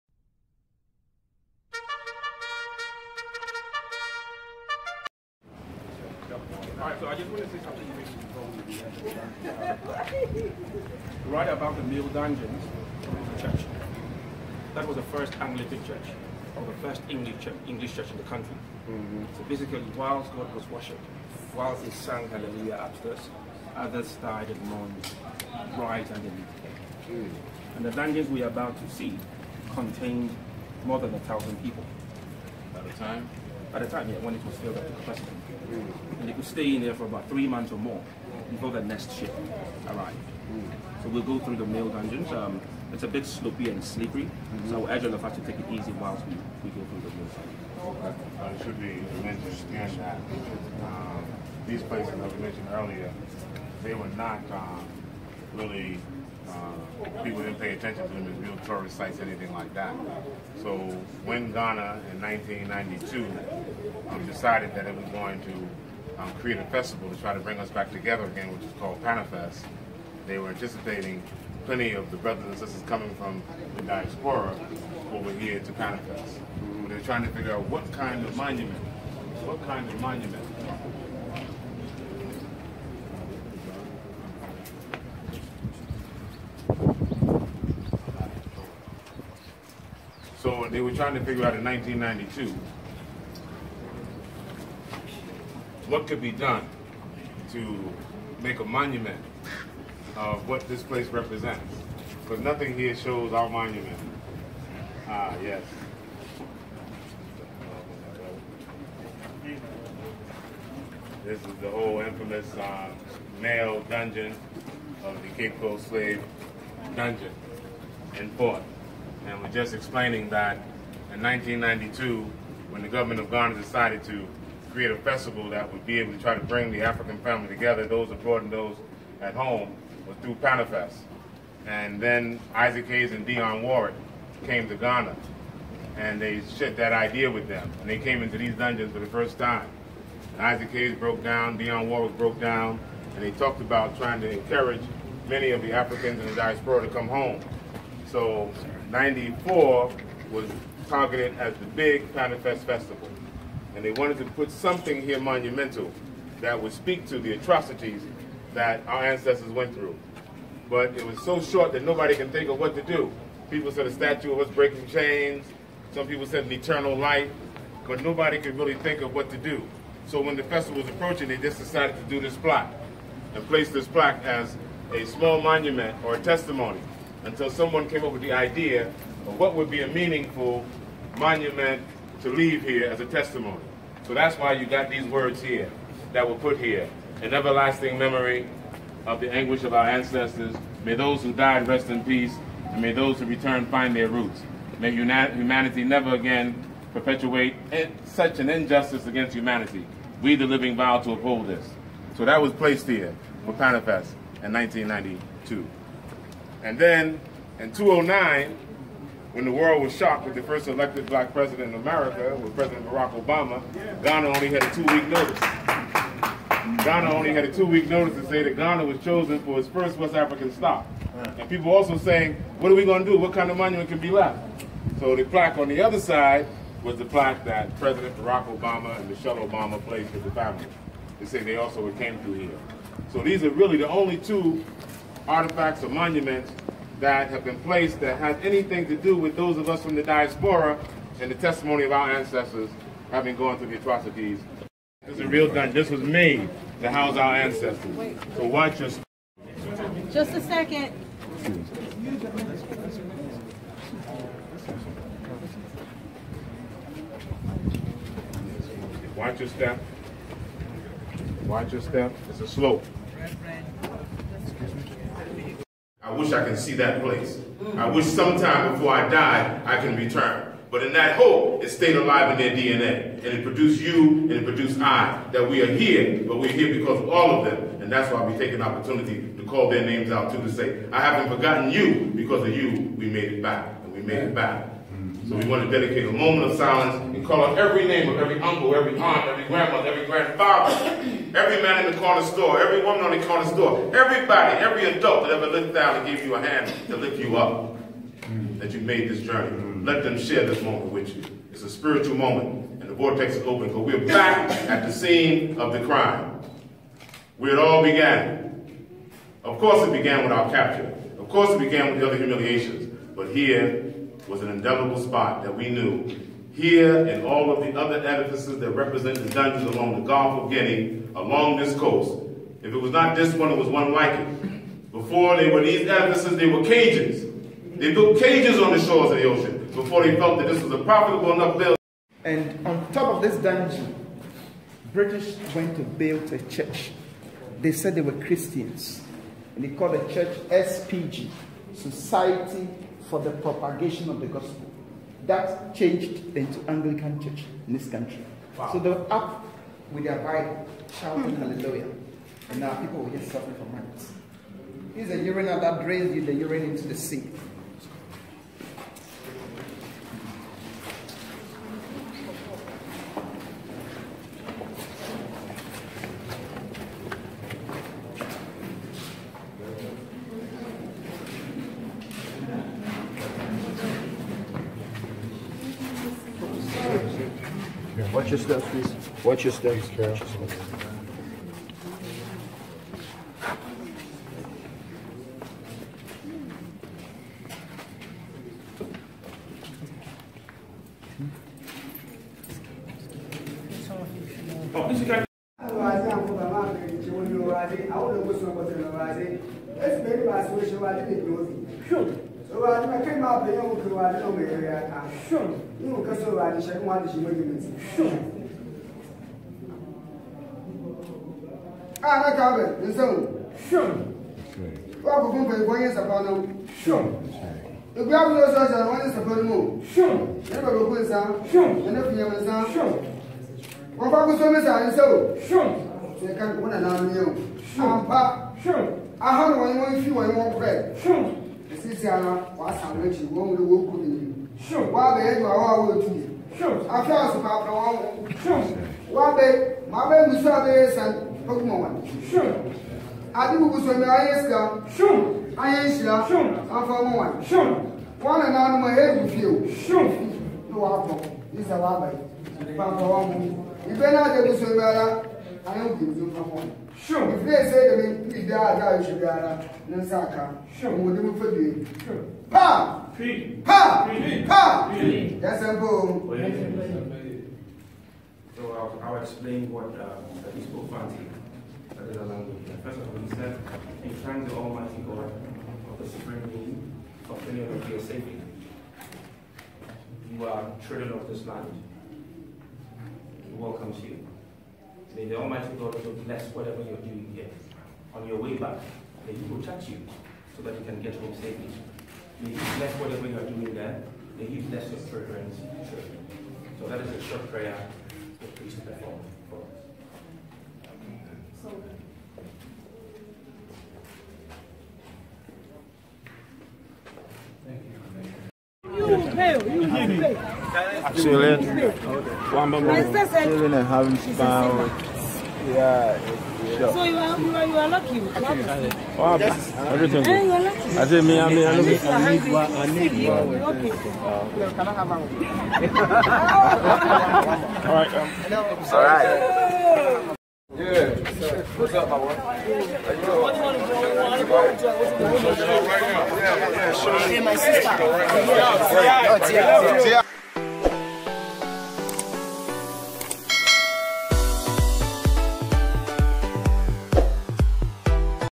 T.I. and Wife Tiny visit Cape coast castle(Ghana) Dr. Boyce Watkins, David Miller and Kendall white.
Clifford Joseph Harris Jr popularly known as T.I. & TIP, Tiny Harris, Dr. Boyce Watkins, Kendall white & Reginald Wilkerson visit Ghana and made a trip to Cape coast castle Dungeon.

Eric Awotwe (Host & Tour guide)